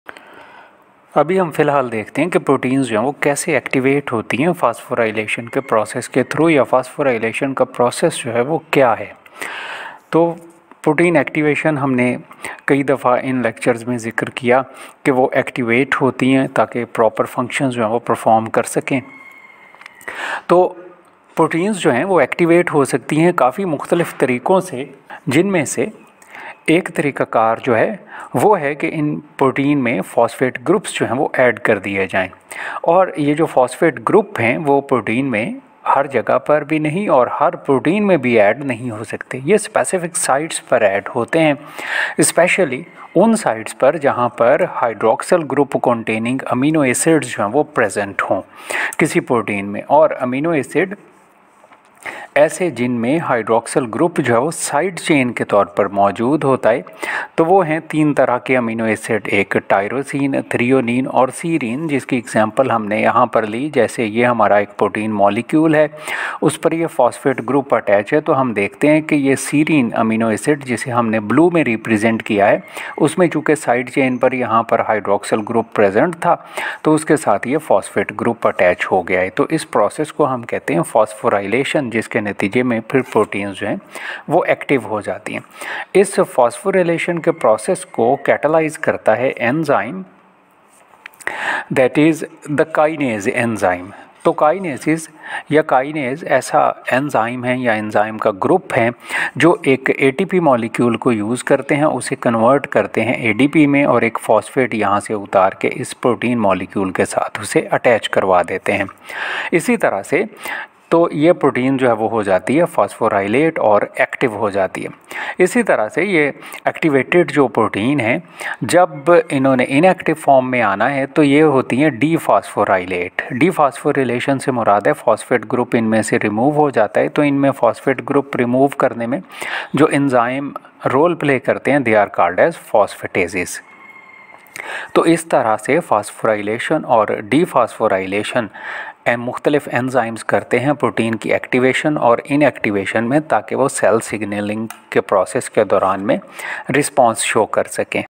Signal Transduction Lecture 02; Phosphorylation or Activation of Proteins
Phosphorylation of proteins, role of Protein kinases and phosphatases in phosphorylation and de phosphorylation